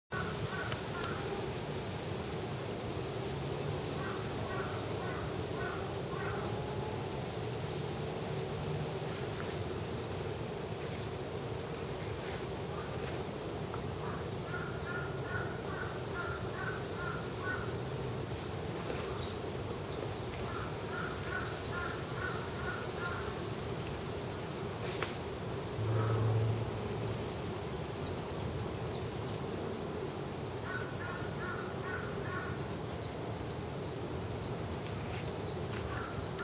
ant mimicry,Myrmarachne japonica
アリグモがうろうろするだけの動画